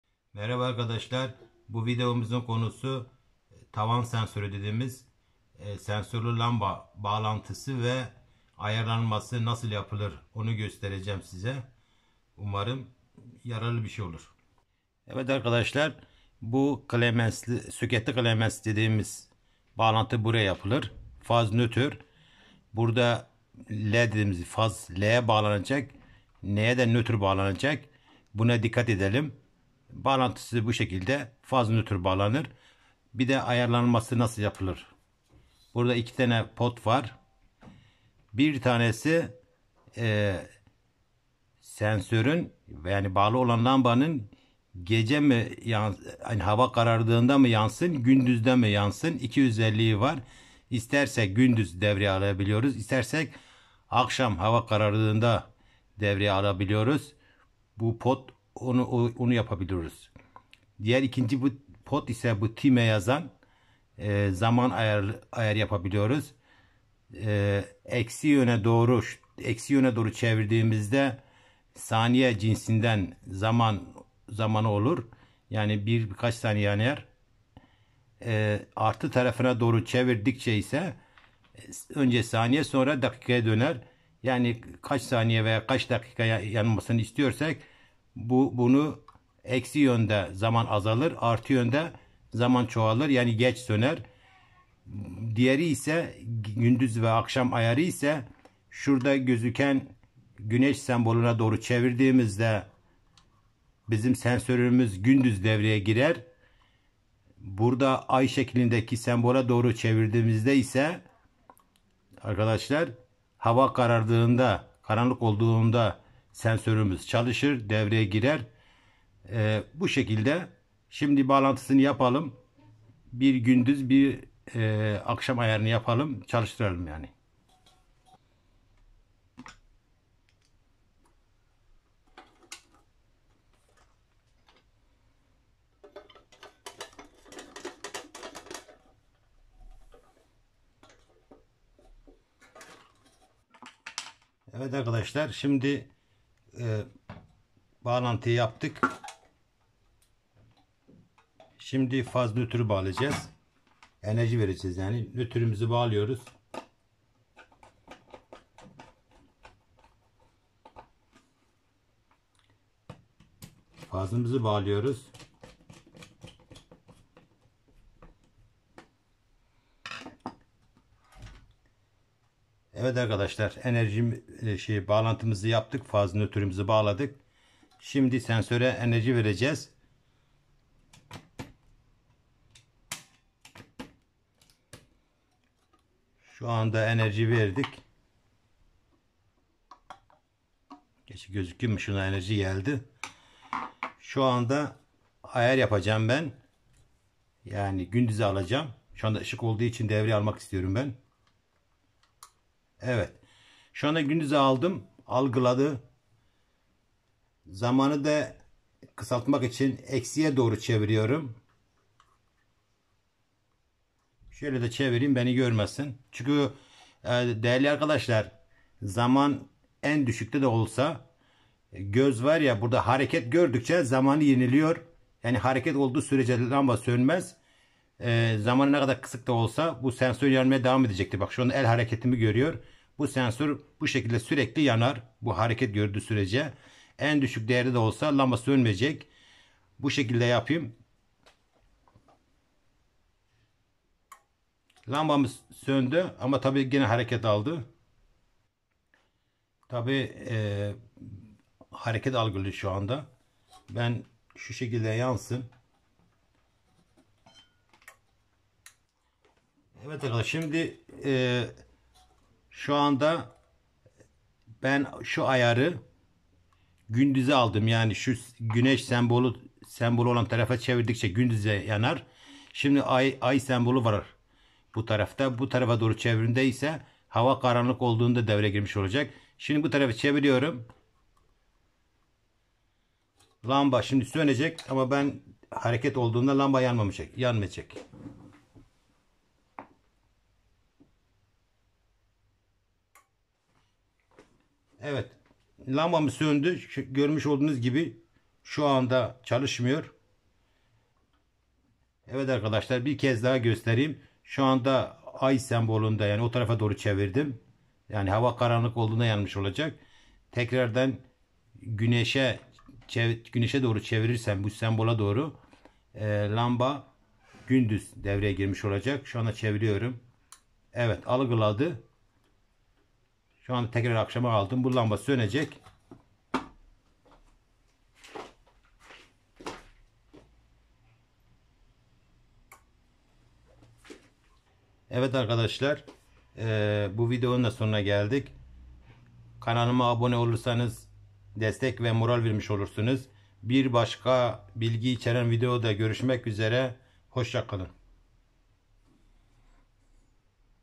Sensörlü Lamba ayarı Nasıl Yapılır?
merhaba arkadaşlar sensörlü lamba ayarı pratik ve kolay bir şekilde nasıl yapılır anlatmaya çalıştım